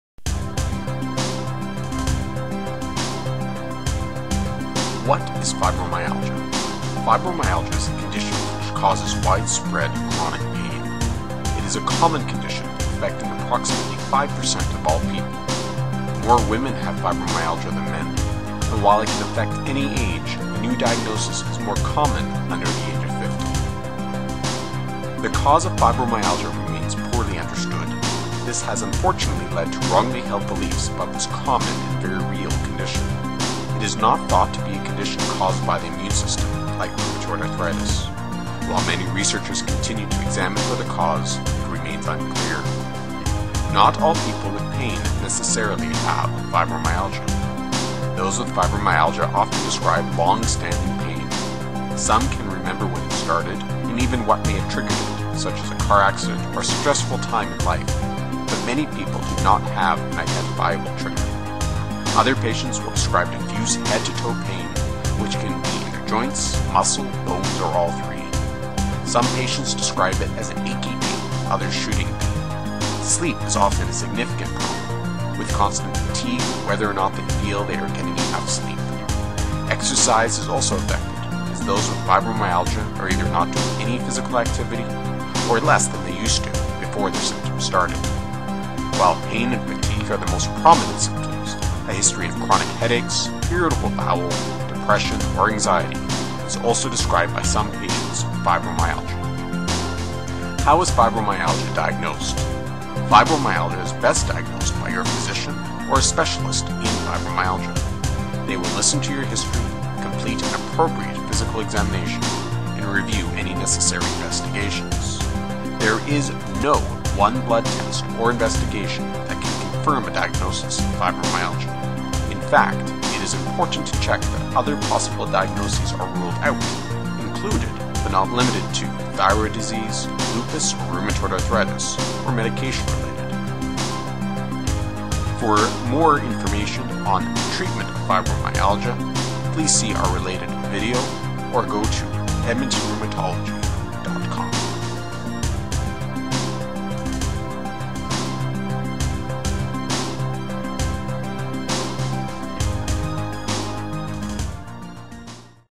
Fibromyalgia: An Overview
What is Fibromyalgia?
How is Fibromyalgia Diagnosed?
This brief video from rheumatologists will answer these questions.